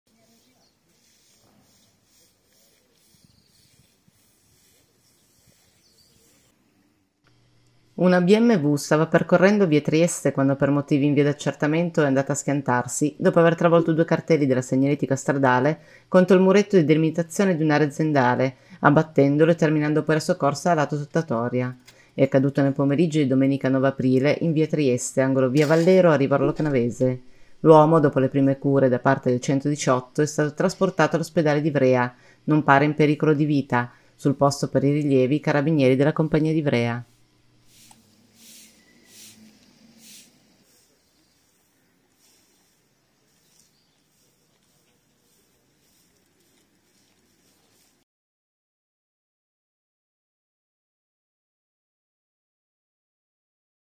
RIVAROLO CANAVESE – Auto si schianta contro ad un muretto in via Trieste
Una Bmw stava percorrendo via Trieste quando, per motivi in via d’accertamento, è andata a schiantarsi, dopo aver travolto due cartelli della segnaletica stradale, contro il muretto di demilitazione di un’area aziendale, abbattendolo e terminando poi la sua corsa a lato rotatoria.

È accaduto nel pomeriggio di oggi, domenica 9 aprile 2023, in via Trieste angolo via Vallero a Rivarolo Canavese.